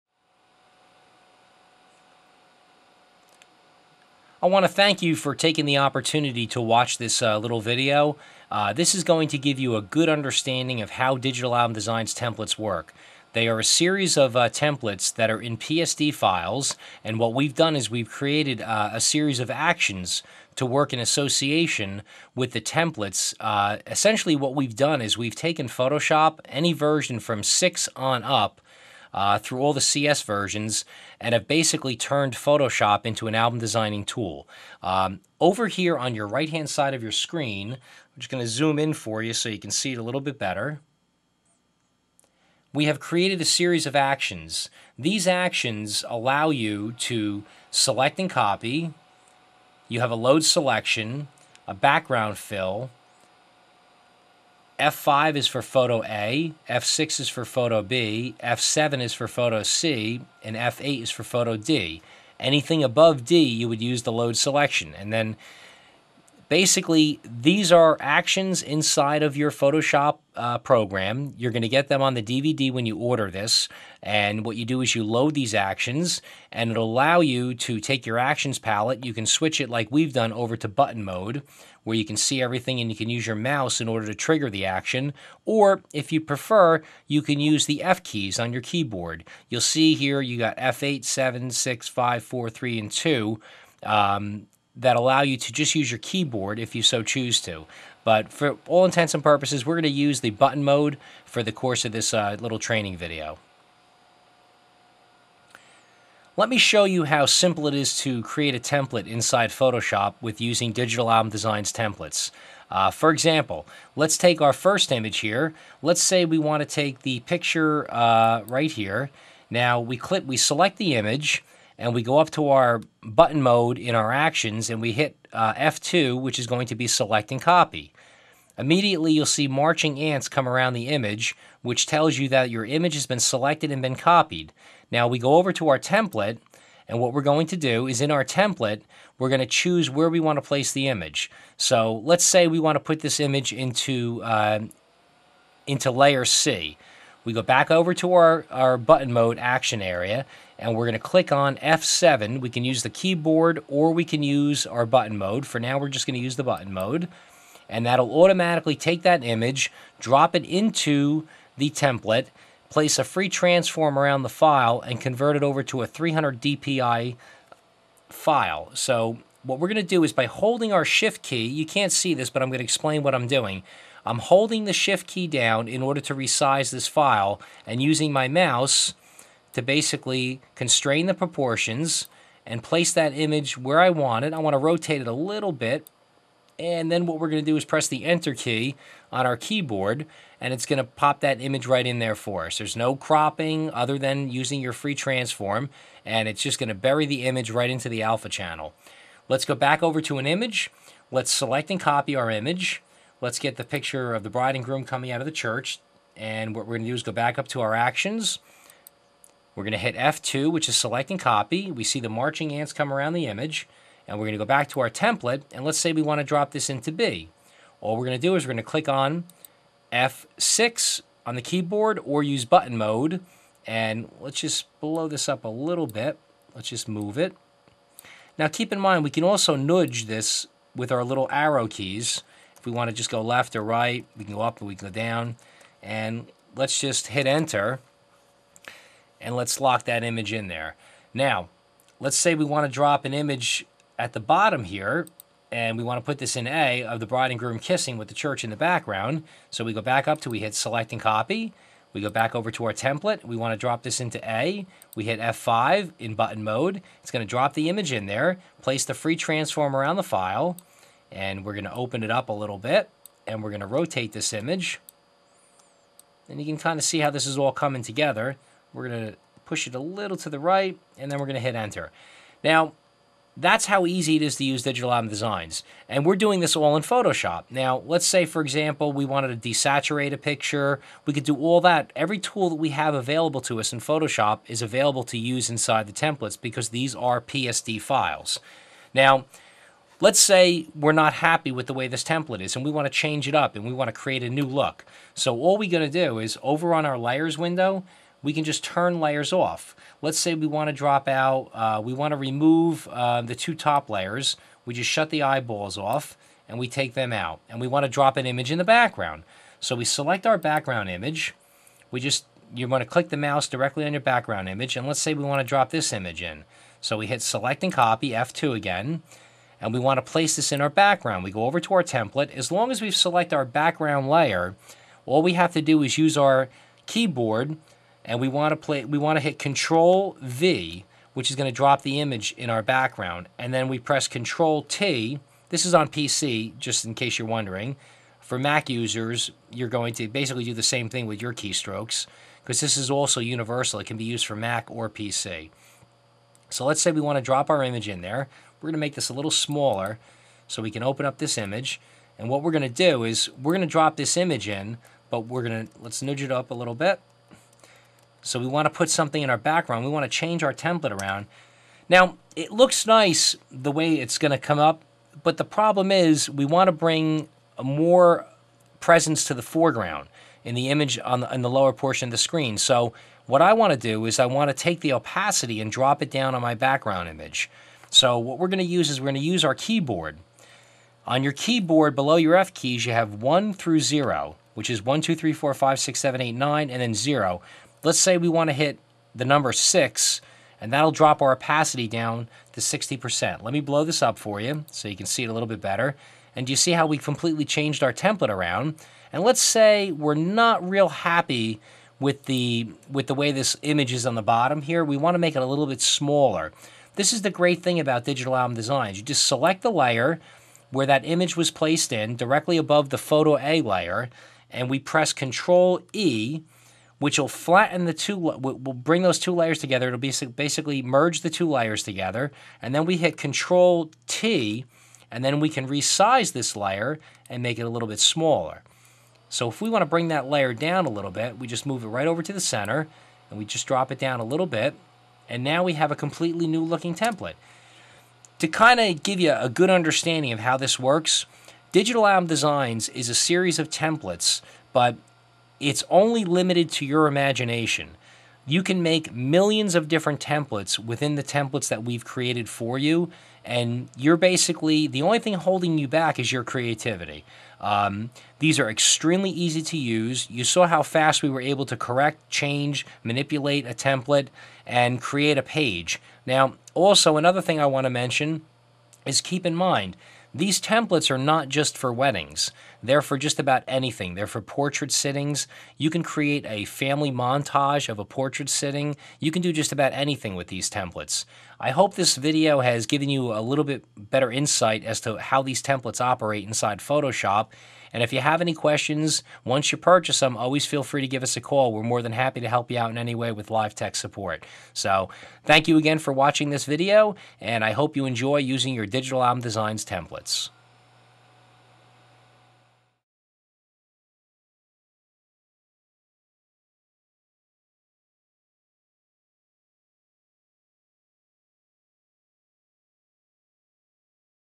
Digital Album Designs
New! Photoshop Actions for Digital Album Design Templates, the actions will speed up the design time for every photographer. These new actions will turn Photoshop into an album designing program.
Photographers will no longer have to look for supplemental programs when designing wedding albums because Digital Album Designs has created the ultimate solution.